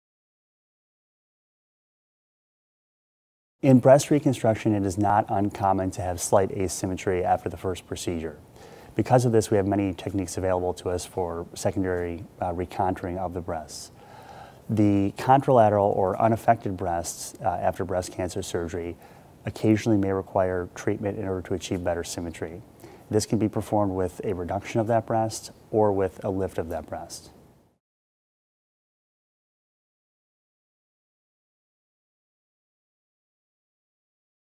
Do patients have surgery on other breast to match? (Patrick Hettinger, MD)
Patrick Hettinger, MD, plastic and reconstructive surgeon, describes the secondary surgical options available to match breast symmetry after reconstruction.

Dr. Hettinger is part of the Froedtert & the Medical College of Wisconsin Breast Reconstruction Program in Milwaukee, Wis.